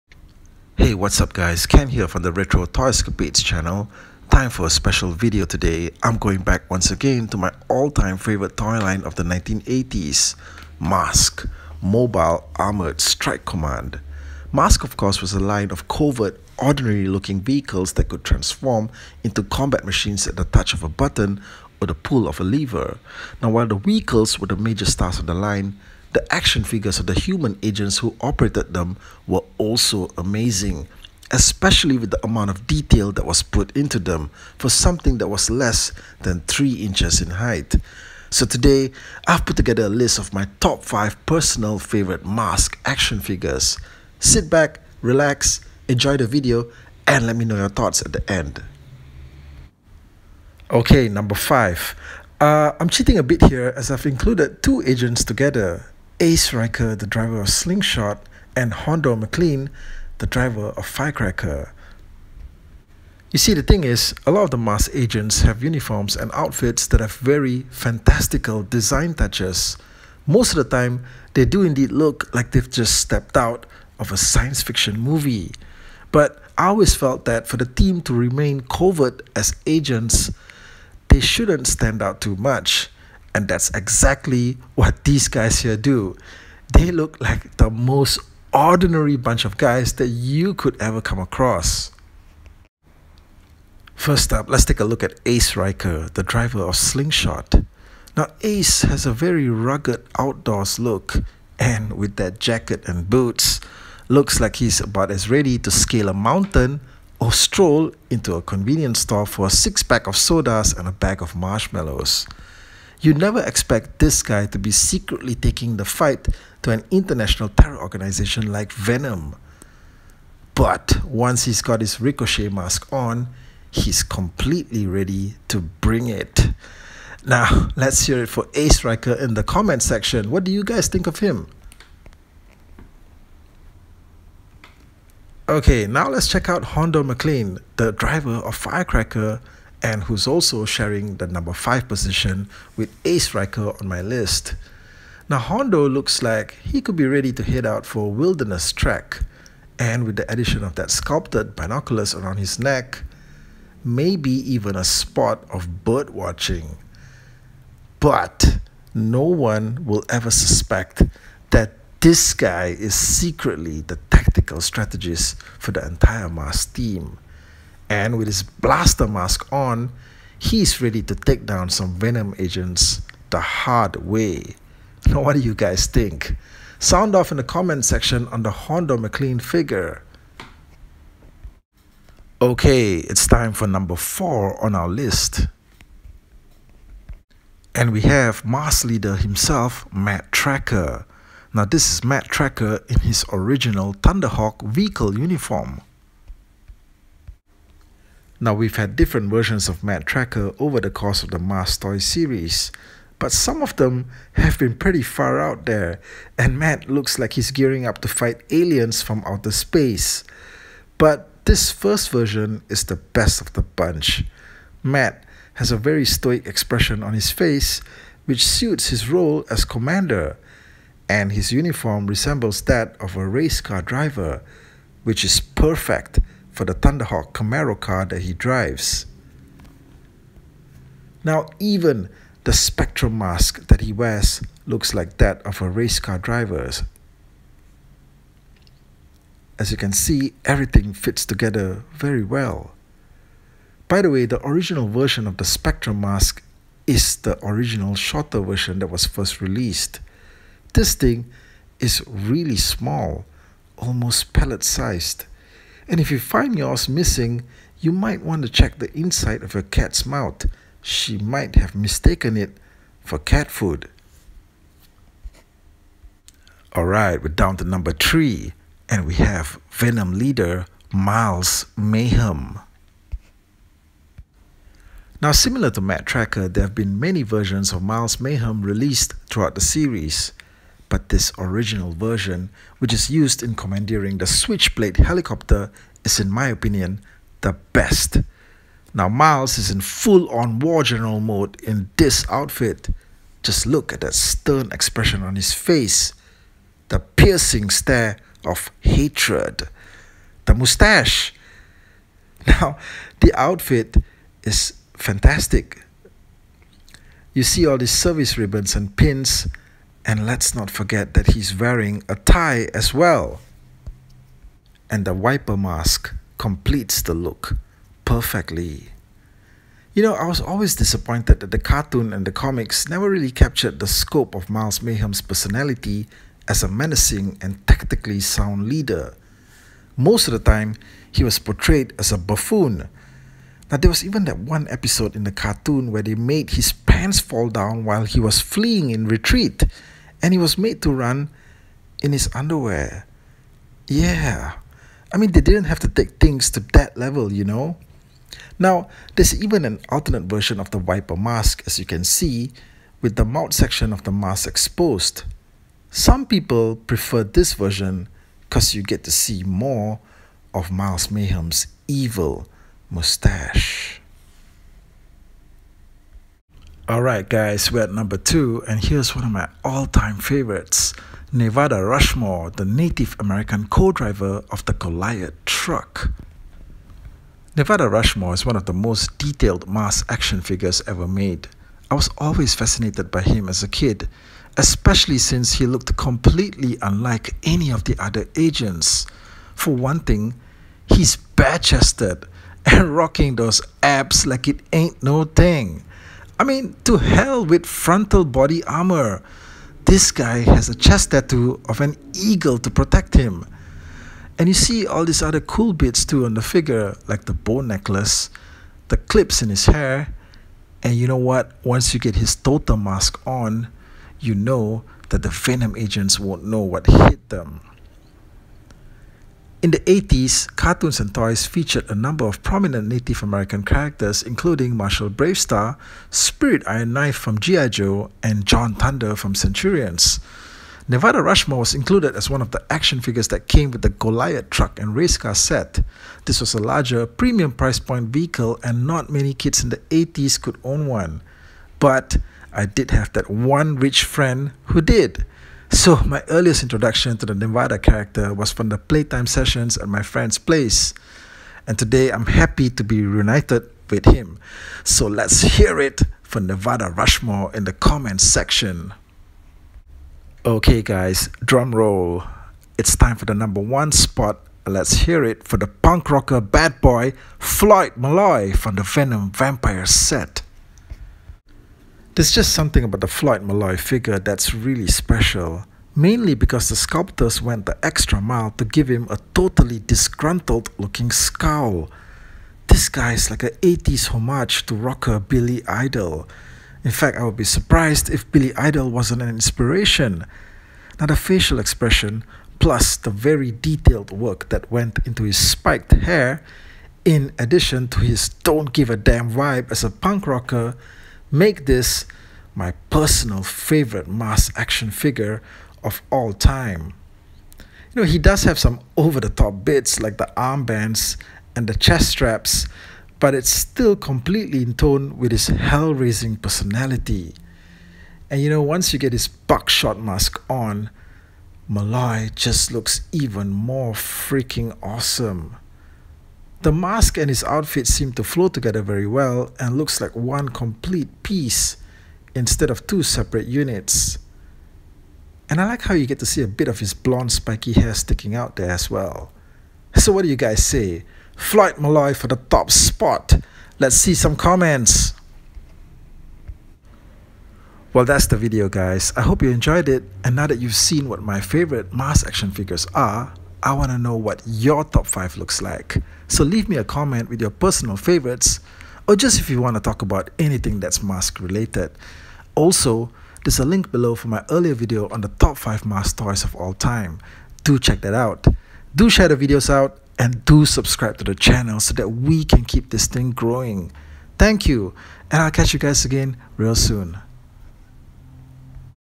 TOP 5 FAVOURITE M.A.S.K. ACTION-FIGURES - A Retro Toy Escapades Special
A list of my Top 5 Favourite M.A.S.K. ACTION-FIGURES of ALL-TIME! These are my personal choices – and I want you guys to share your own list in the comments!

M.A.S.K. TOYS - The Top 5 M.A.S.K. TOYS of ALL-TIME!